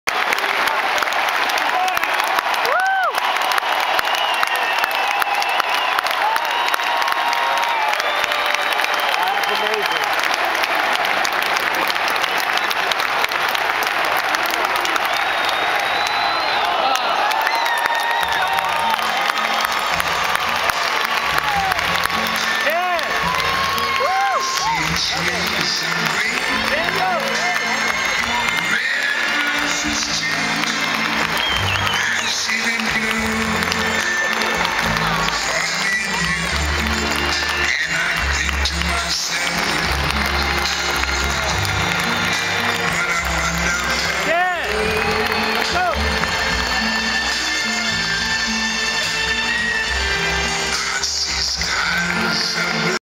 Shea Goodbye - 09/28/2008 Last Pitch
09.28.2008 - Tom Seaver pitches to Mike Piazza, last pitch in Shea Stadium.. An emotional moment.